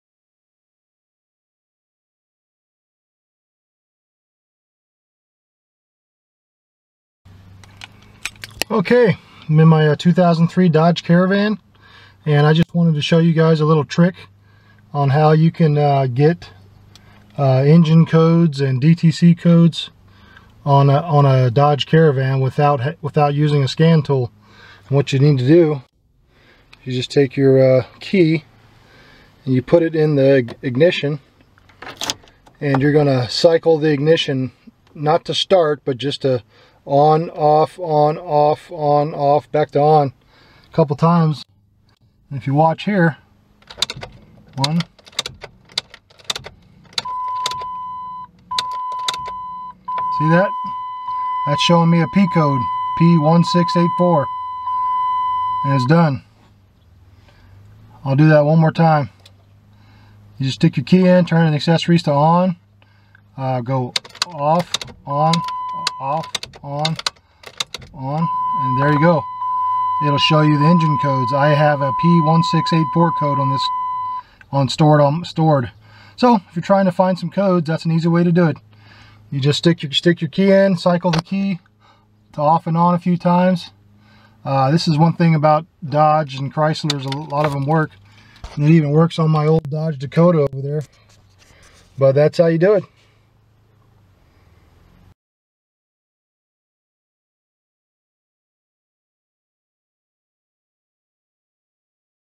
How To Get DTC Codes Without Using A Scan Tool
In this video I will show you how to get codes off a vehicle without using a scan tool or code reader. This trick works on a lot of Dodge and Chrysler vehicles. I am demonstrating this trick on my 2003 Dodge Caravan,